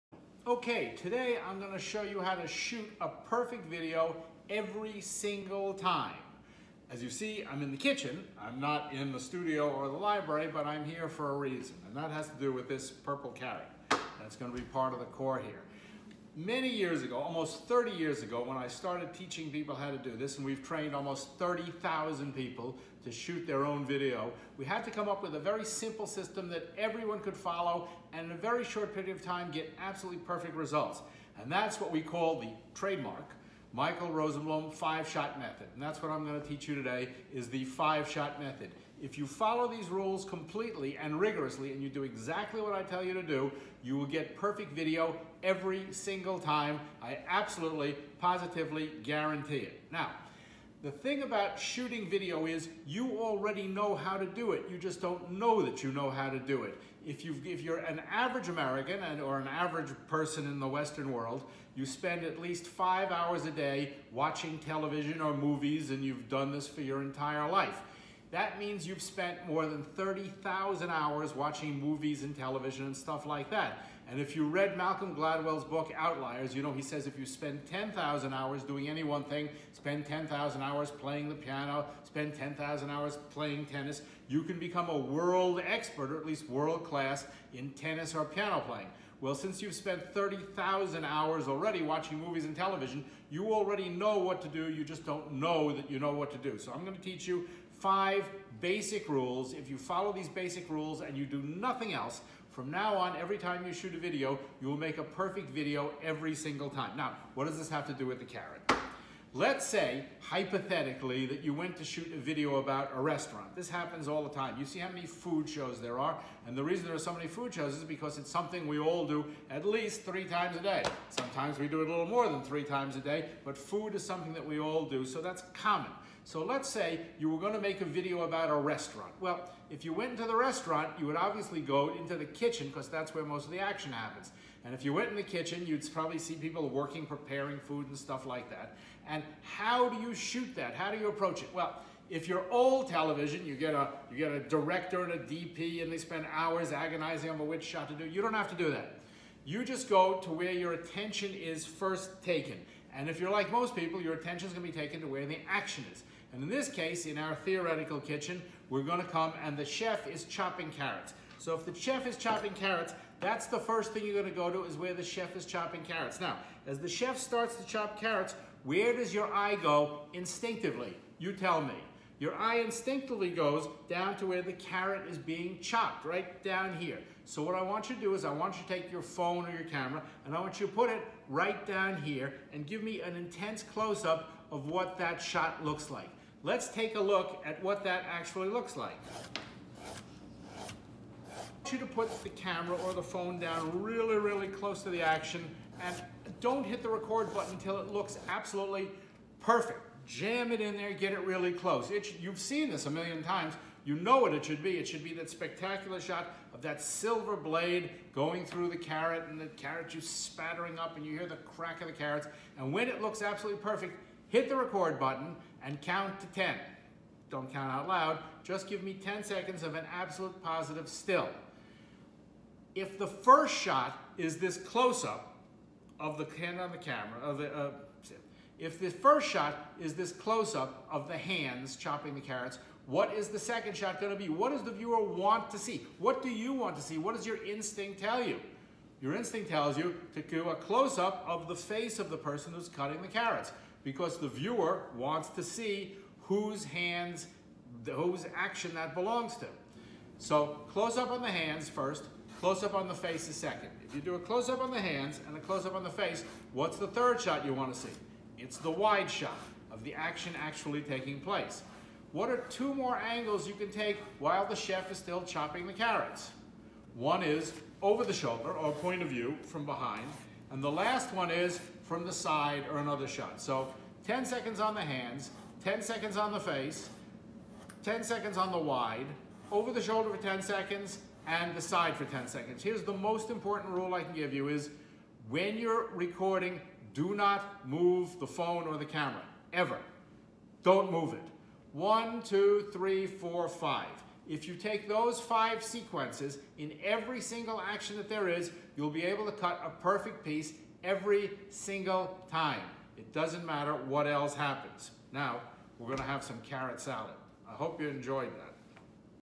How To Shoot A Perfect Video Every SIngle Time
The Michael Rosenblum 5-Shot Method.  If you do this, and nothing but this,over and over again, you will be able to shoot absolutely perfect videos time after time.  The simplest way to make perfect video.

Guaranteed.

If you like what you see here, check out our online Video/Film school